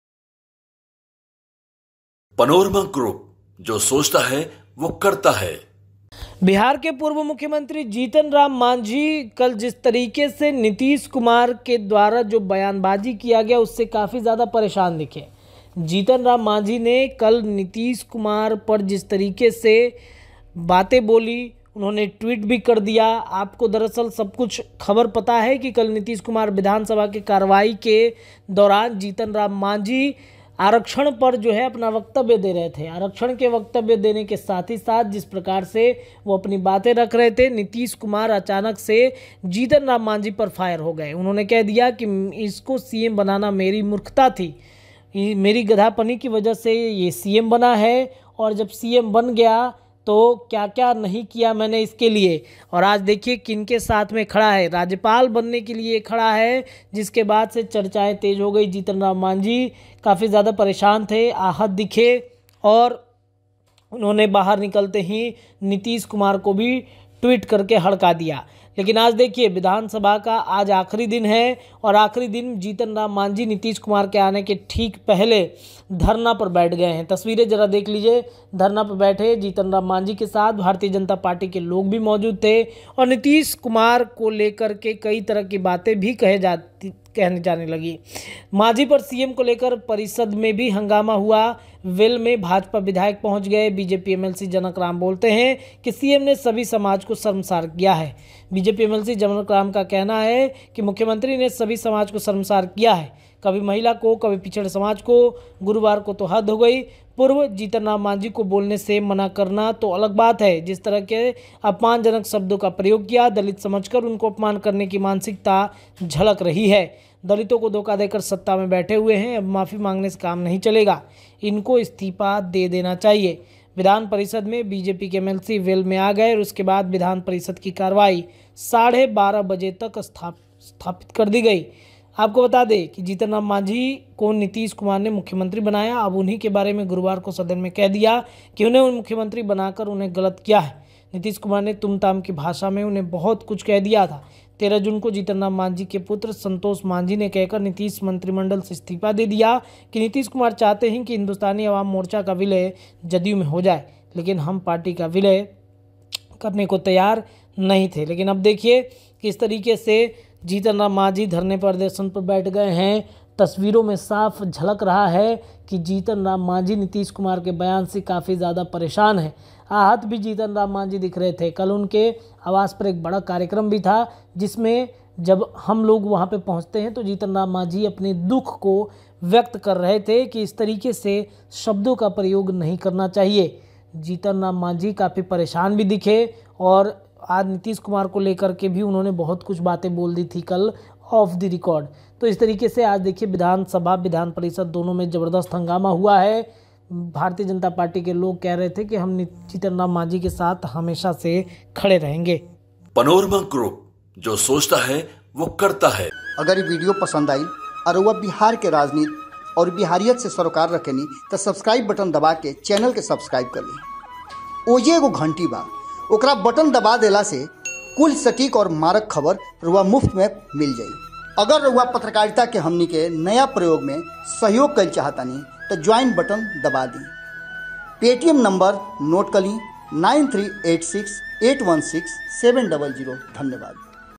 Big Breaking ! नीतीश के सामने ही धरने पर बैठ गए जीतन राम मांझी बोले माफ़ी मांगे नीतीश अपमान का...
Big Breaking ! नीतीश के सामने ही धरने पर बैठ गए जीतन राम मांझी बोले माफ़ी मांगे नीतीश अपमान का बदला नहीं सहेंगे